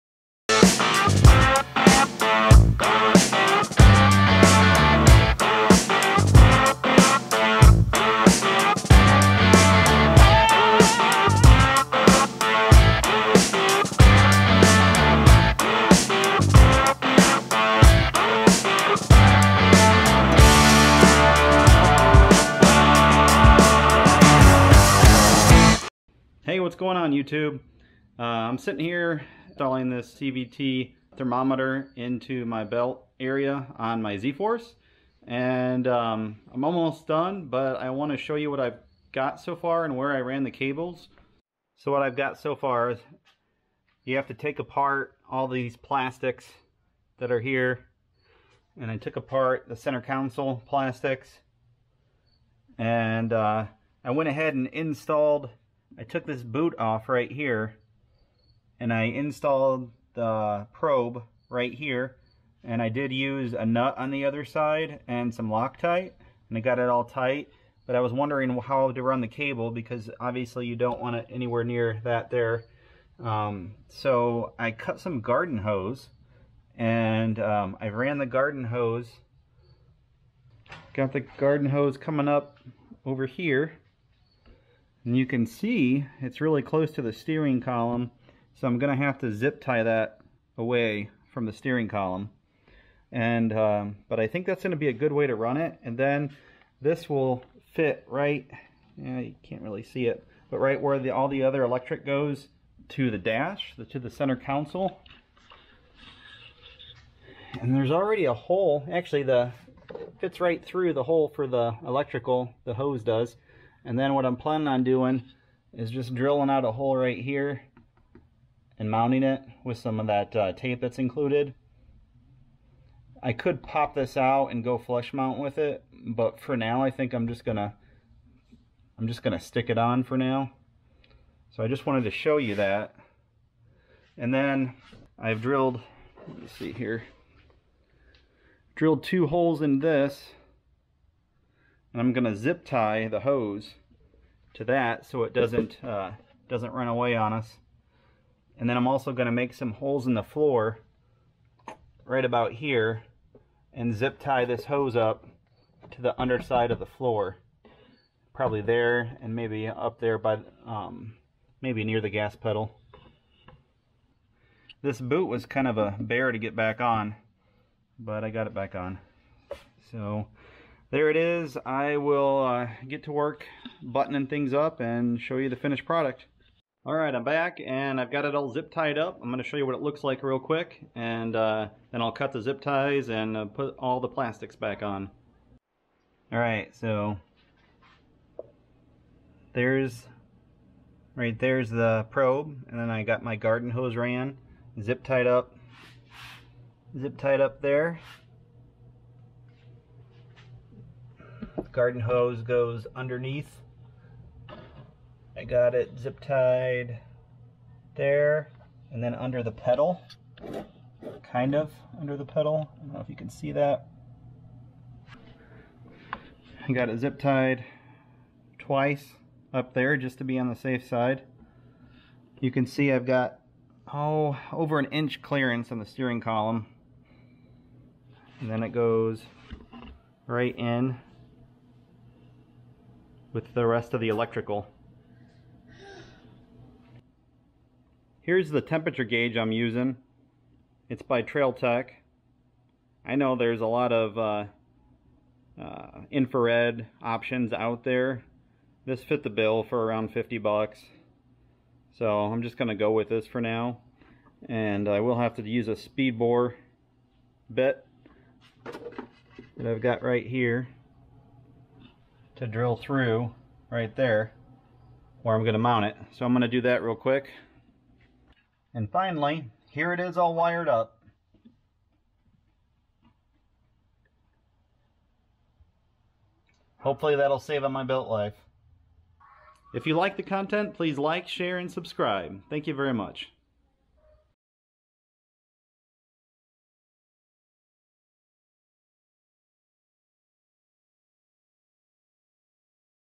CVT Temp Gauge Install on ZForce
Cable routing for CVT temp gauge install on a Cfmoto Zforce.